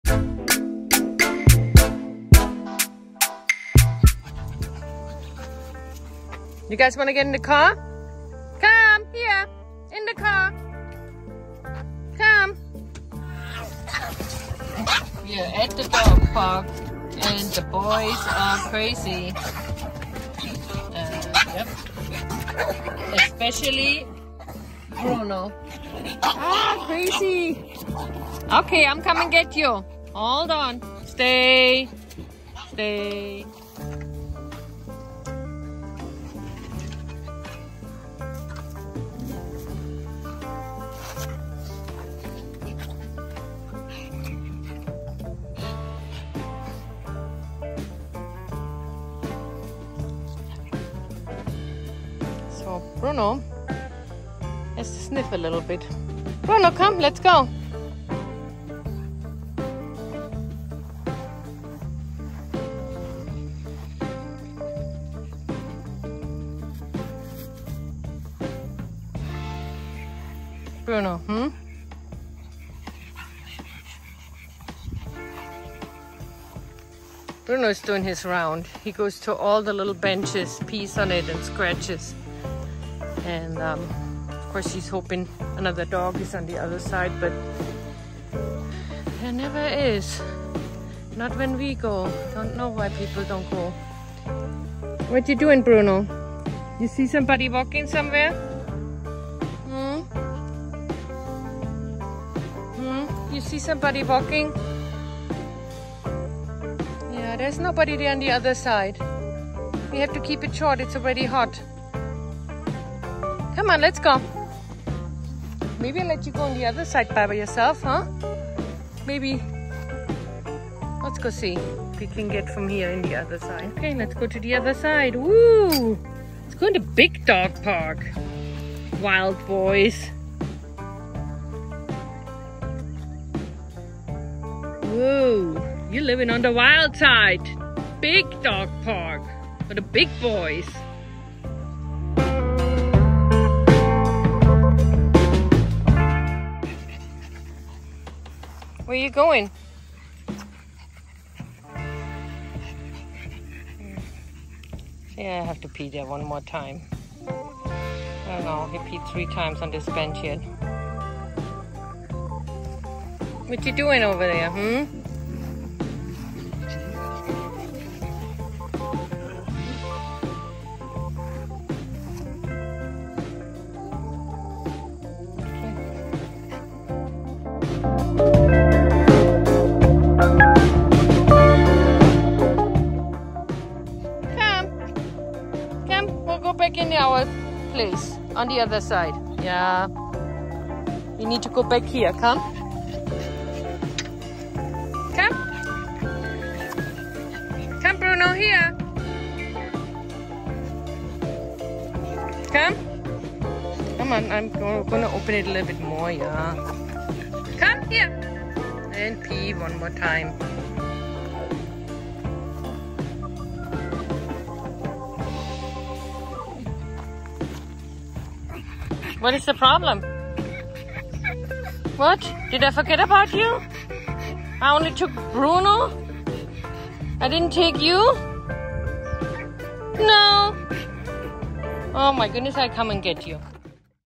Pug‘s just wanna have fun! Where are the dogs?
Poor Bruno! Every time we go to the dog park no dogs are there.